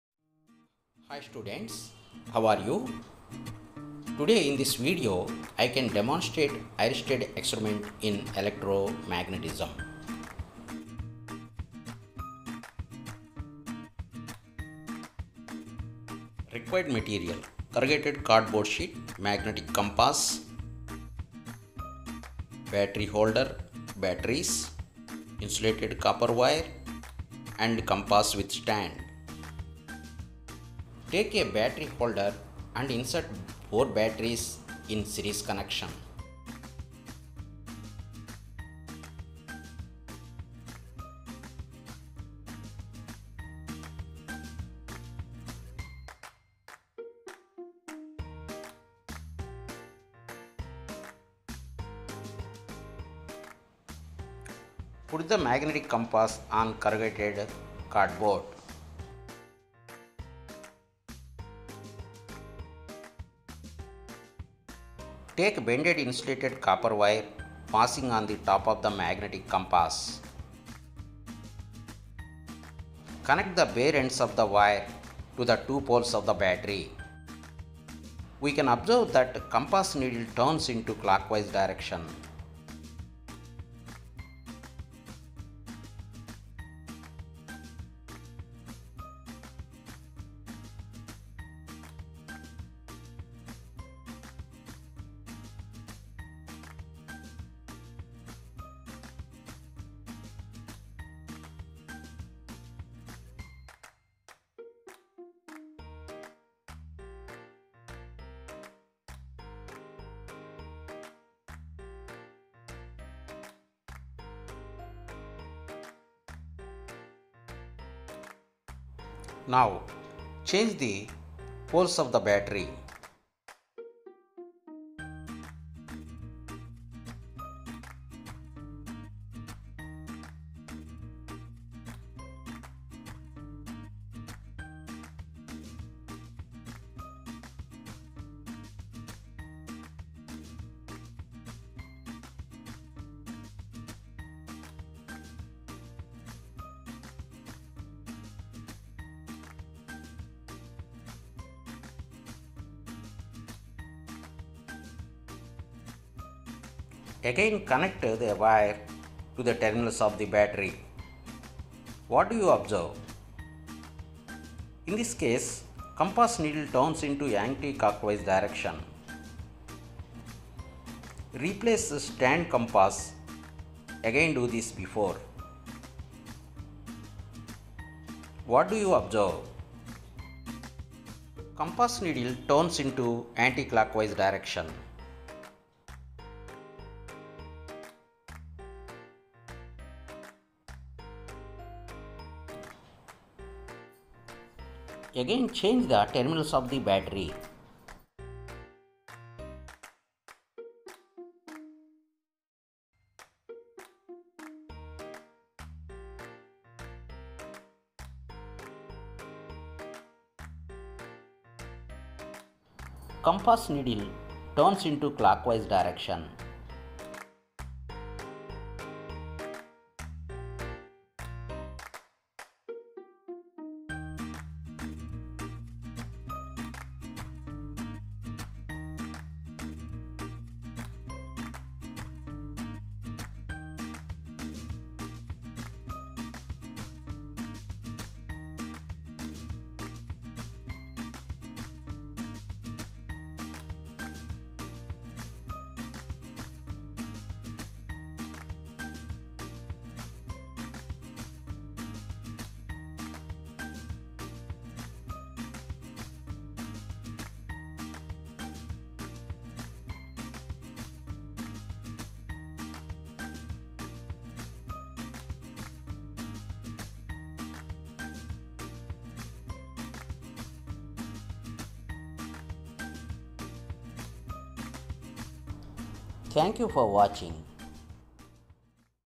OERSTED EXPERIMENT
In this video I can demonstrate Oersted Experiment.  Hans Christian Oersted was one of the leading scientist in the 19th century, played a crucial role in electromagnetism.  He placed a compass needle underneath a wire and then turned on electric current.  The needle of compass showed deflection.  I can show this phenomena in this video.